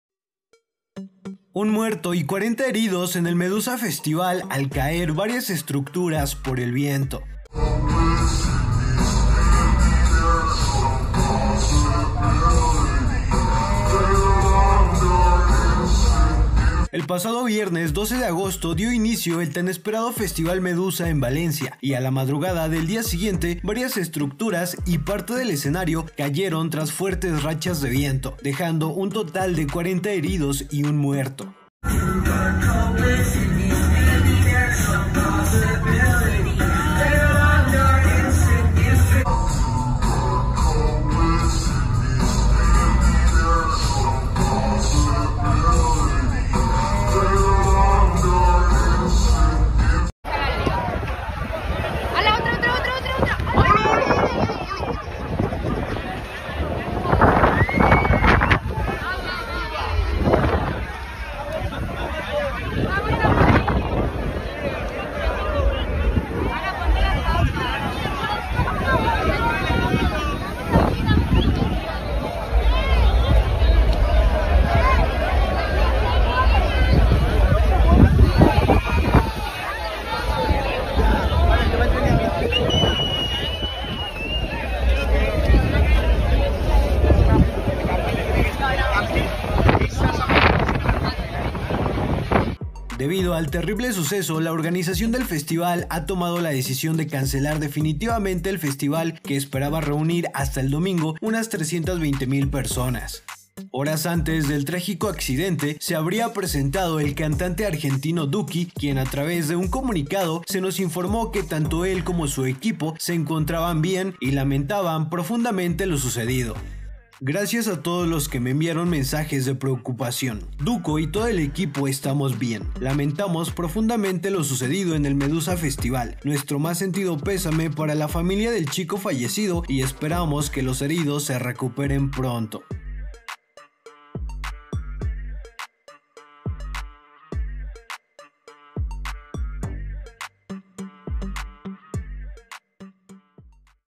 TRÁGICO ACCIDENTE en el MEDUSA FESTIVAL donde había tocado DUKI
El Pasado Viernes 12 de Agosto dio Inicio el tan esperado FESTIVAL MEDUSA en Valencia, y a la madrugada del día siguiente varias estructuras y parte del escenario cayeron tras fuertes rachas de viento, dejando un total de 40 Heridos y 1 Muerto.

Debido al terrible suceso, la organización del festival ha tomado la decisión de cancelar definitivamente el evento que esperaba reunir hasta el domingo unas 320 mil personas.

Horas antes del trágico accidente se habría presentado el Cantante Argentino Duki, quien a través de un comunicado se nos informo que tanto el como su equipo se encontraban bien, y lamentaban profundamente lo sucedido.